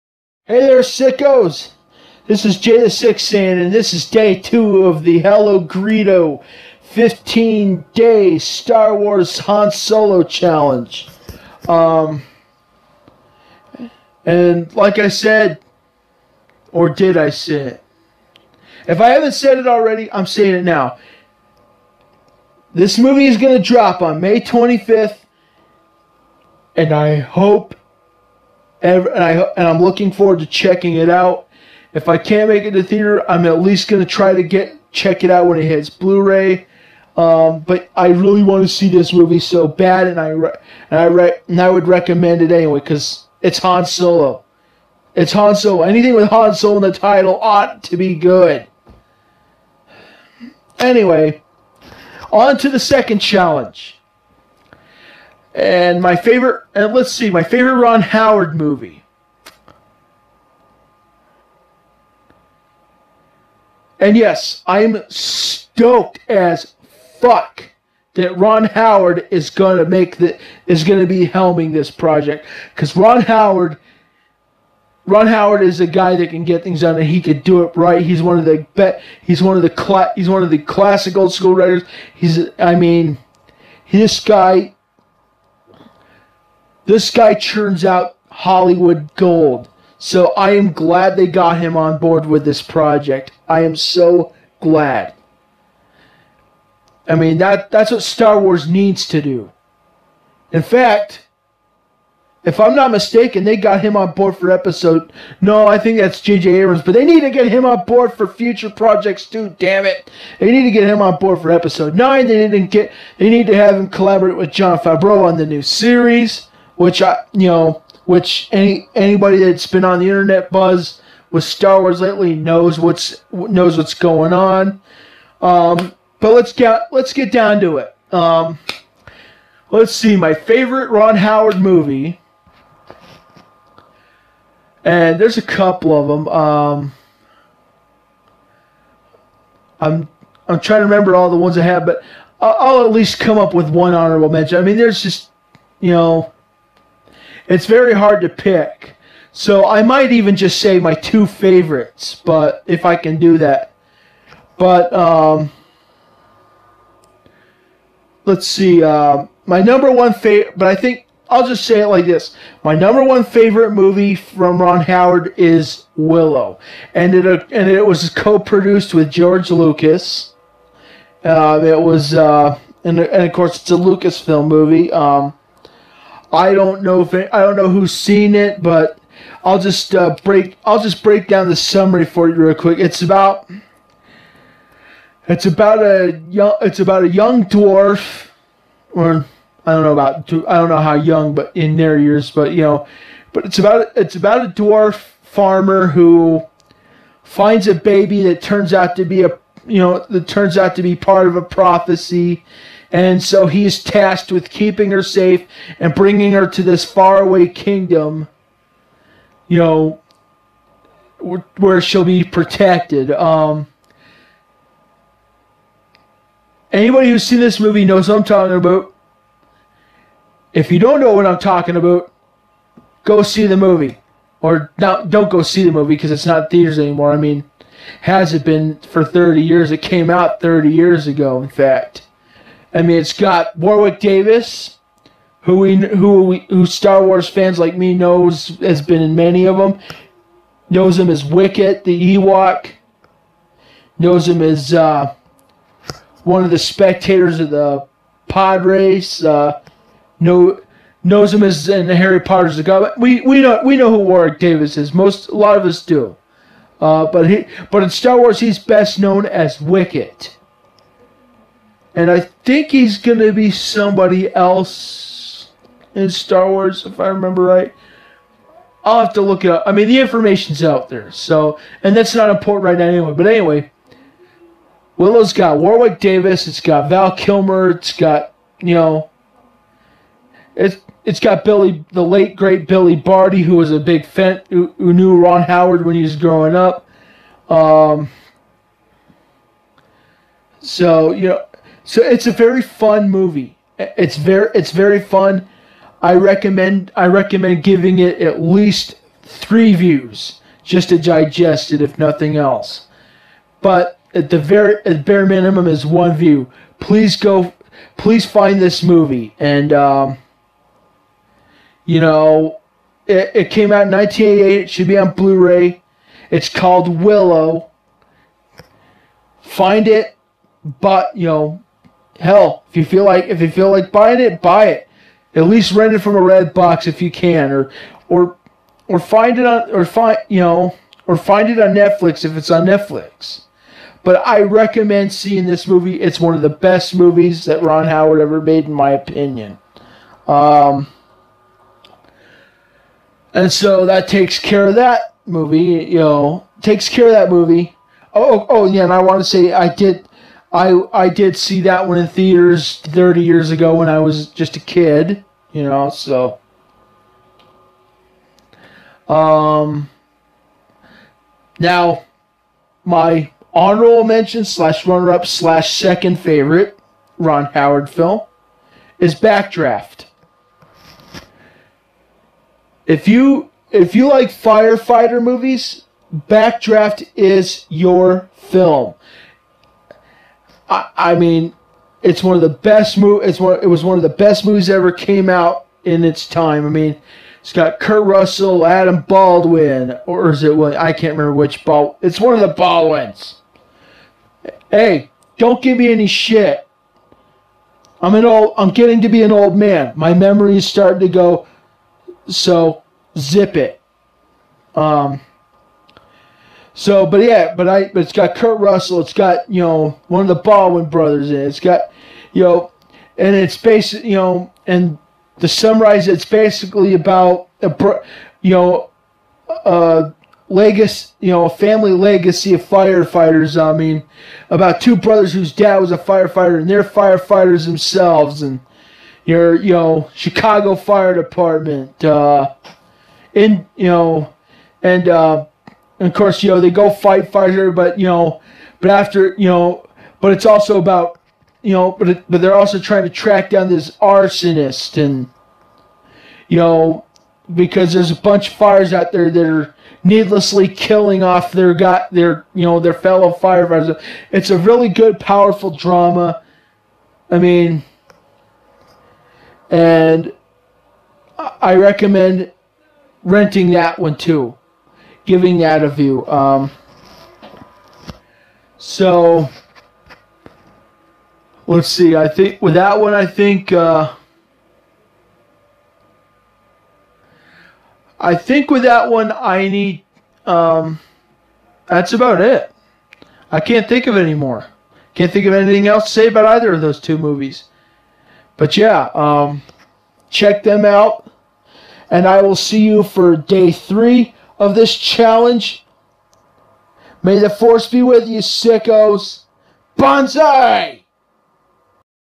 Solo: A Star Wars Story 15-Day Challenge Day 2
In honour of legendary film director Ron Howard being hired to direct his first Star Wars film, the SickSaiyan discusses his favourite Ron Howard film for Day Two of HelloGreedo's 15-Day Han Solo Challenge.

Speaking of my favourite stormtrooper...be sure to check out HelloGreedo's channel, which can be found right here:

This guy puts out great Star Wars content.